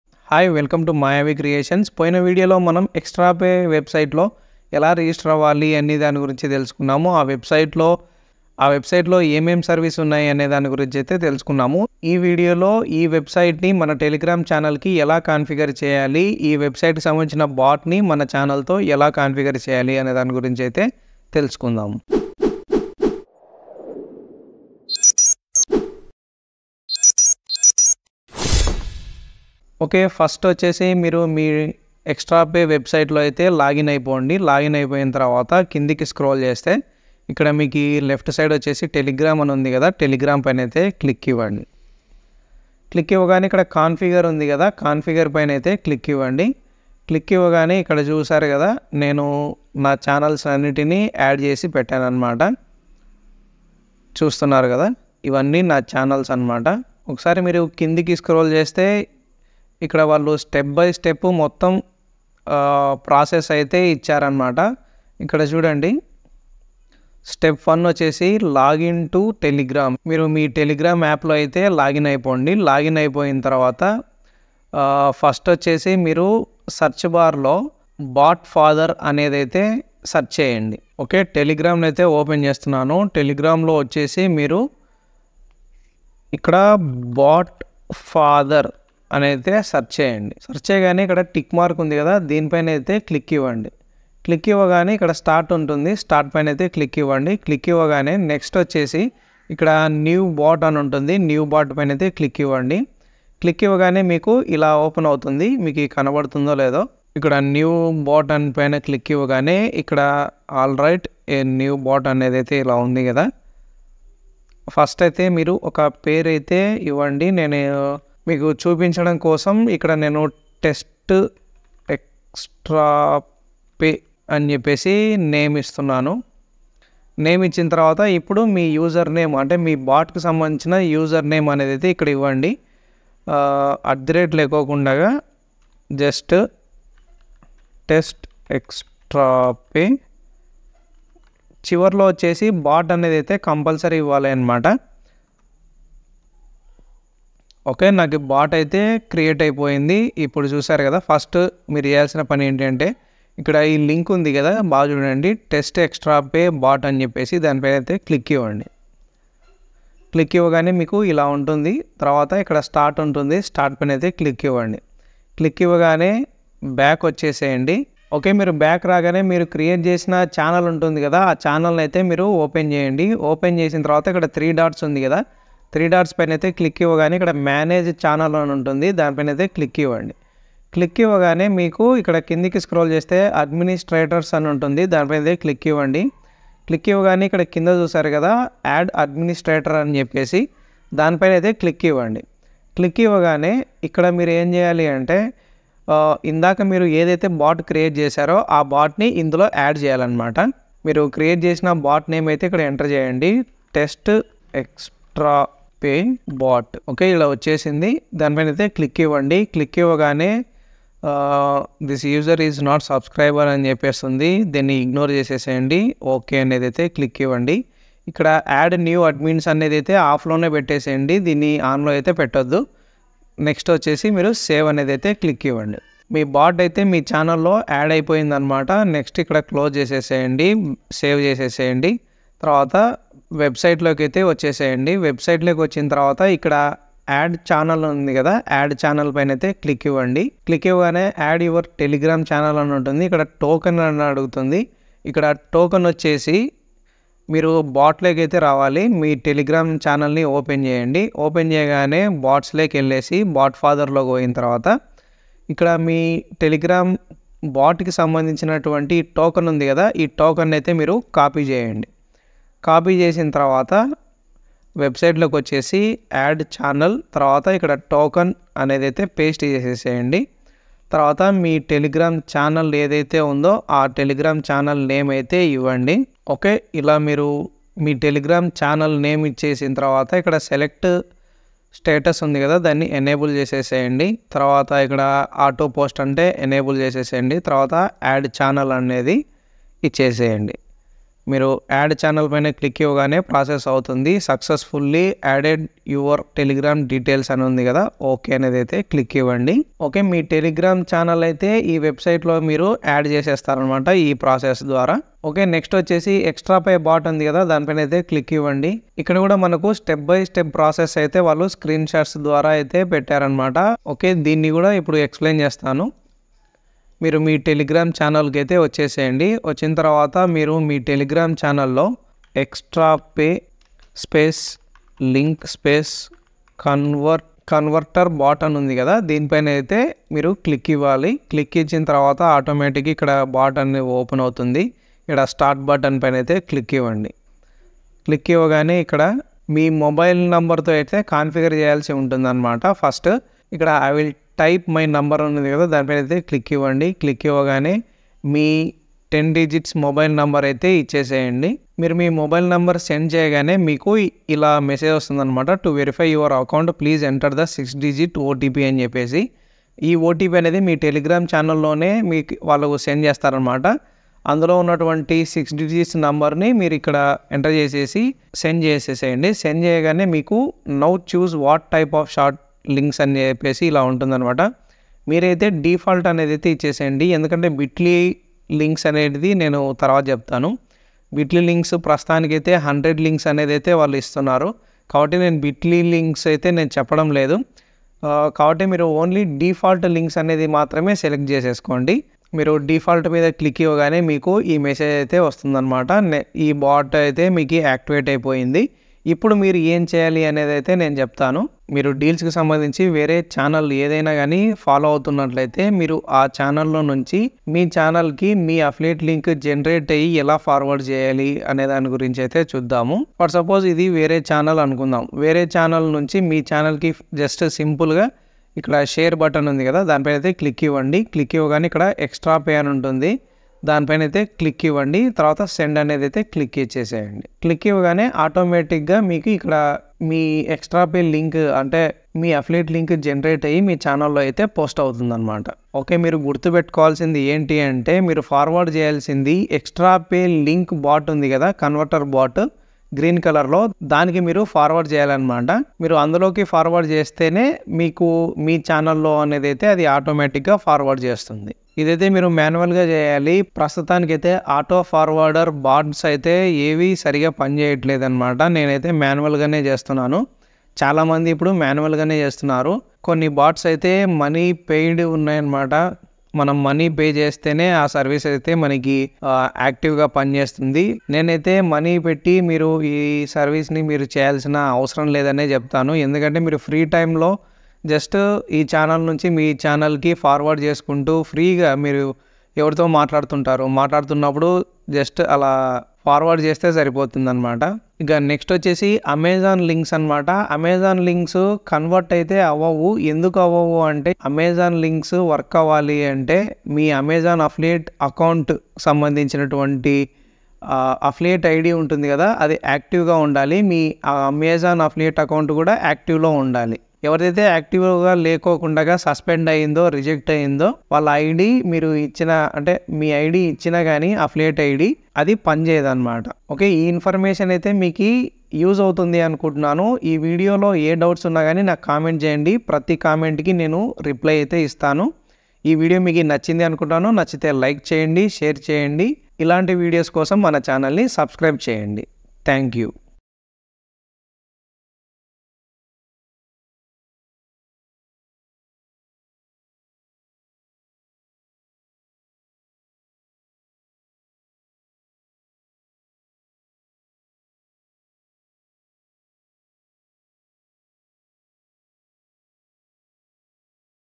How to Configure Extrape Telegram Bot in Telugu | Part 2 | Money Earning Website | Mayavi Creations
Instagram ID 👉 @mayavicreations
MY GEARS
my Mic 🎤
my Laptop 💻
my Tripod Mobile Tablet Stand
my Mouse Pad
my Lighting set